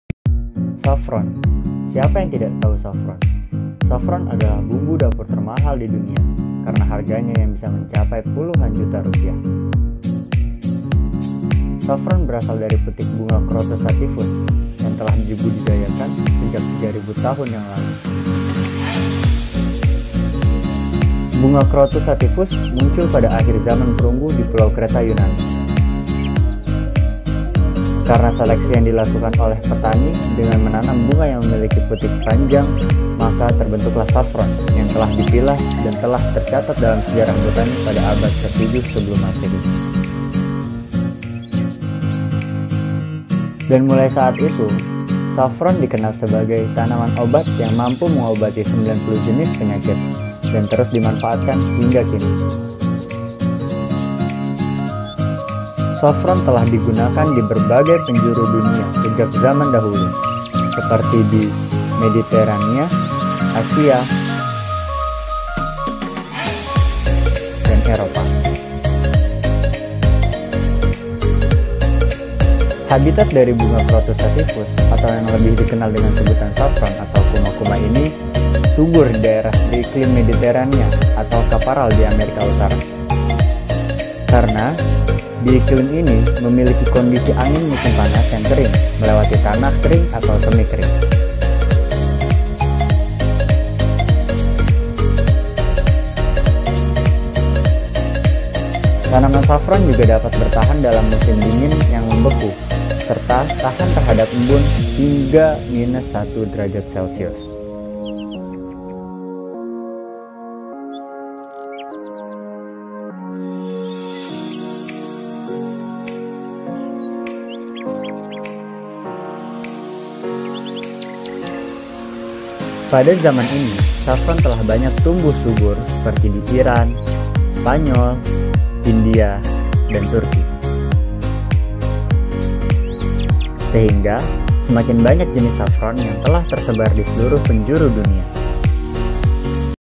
Tahukah Kamu Sejarah Saffron?
Saffron adalah nama untuk rempah-rempah dari bunga Crocus sativus, sekaligus nama umum untuk tanaman Crocus sativus dari marga crocus famili Iridaceae. Bunga kuma-kuma memiliki tiga kepala putik yang terletak distal terhadap daun buah.

Ingin tahu sejarah dari Saffron Seperti apa ?
Simak vidio berikut, dan mari belajar bersama dari manfaat dari bunga sejuta manfaat ini.

Terimakasih